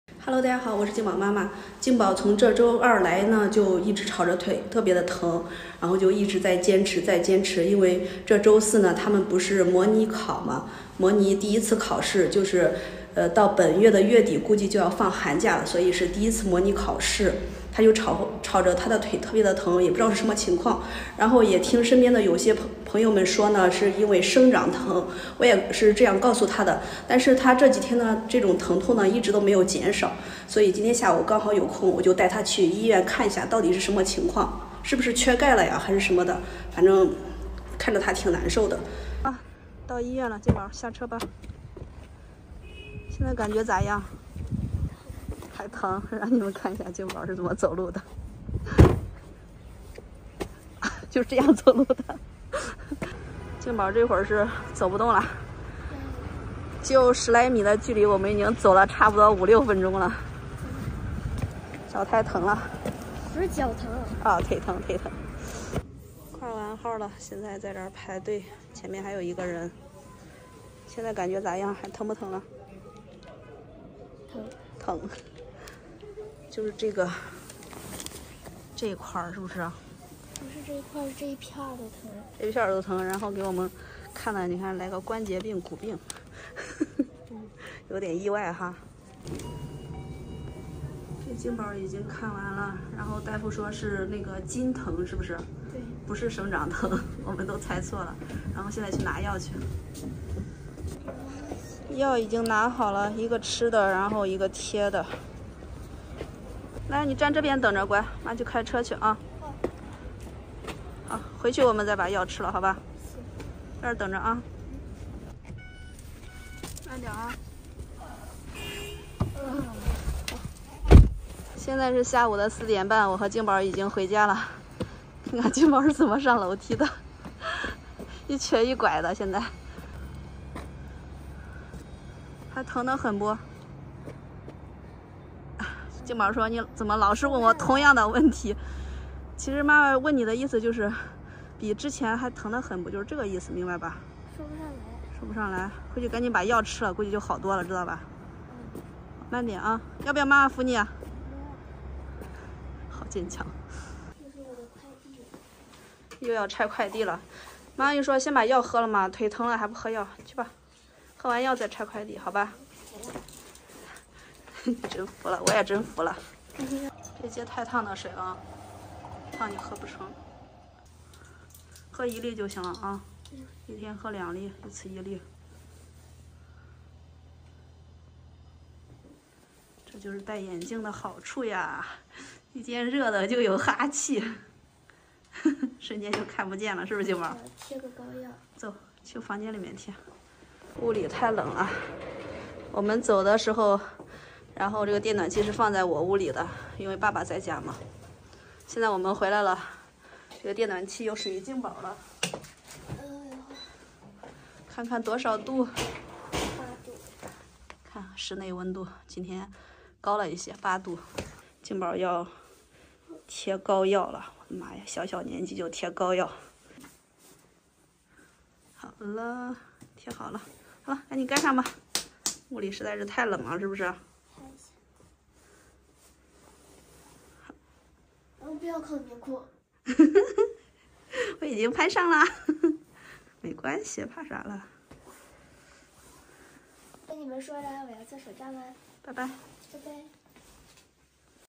静宝的膝盖疼了三四天，妈妈一直认为是生长疼，差点就给耽误了
成为此频道的会员即可获享以下福利：
记录和分享静宝成长路上的点滴，感谢有你们的陪伴。